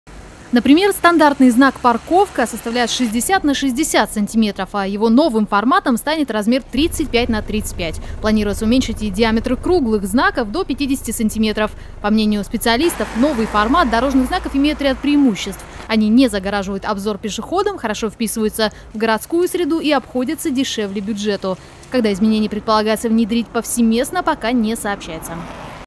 Дорожные знаки в России планируют уменьшить
Внешний вид дорожных знаков в России будет изменен. Об этом пишет сегодня "Российская газета". Стандартный размер знаков в ряде случает планируется уменьшить в два раза. Это инициатива ГИБДД.